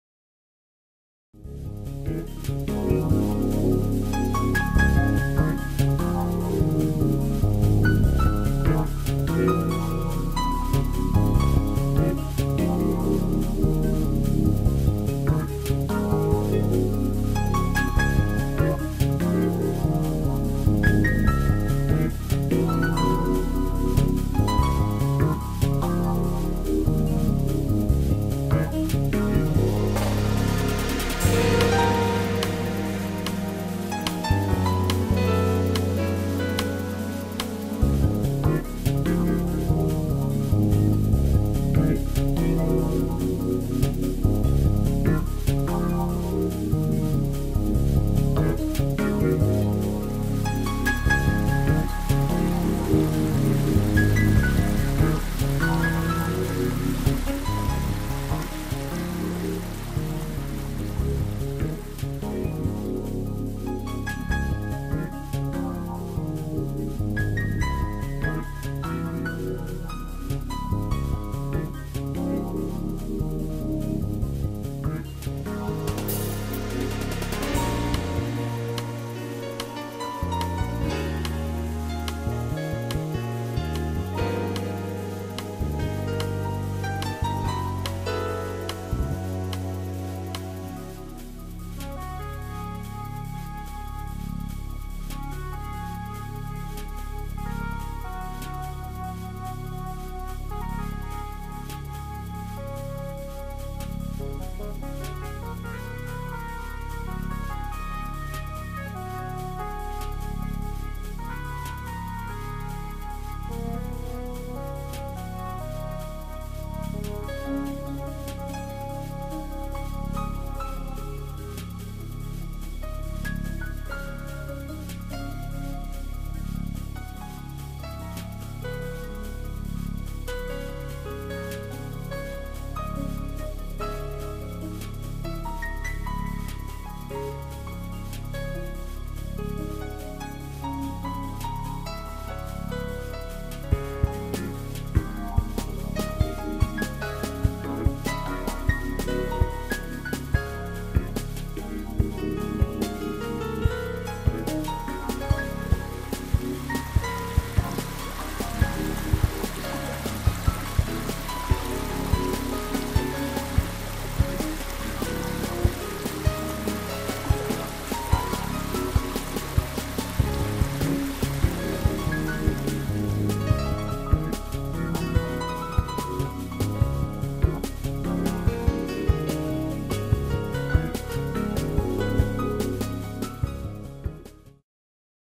Progetto del termovalorizzatore 3d
Animazione 3d del termovalorizzatore realizzato dallo studio Bertone. (dicembre 2008)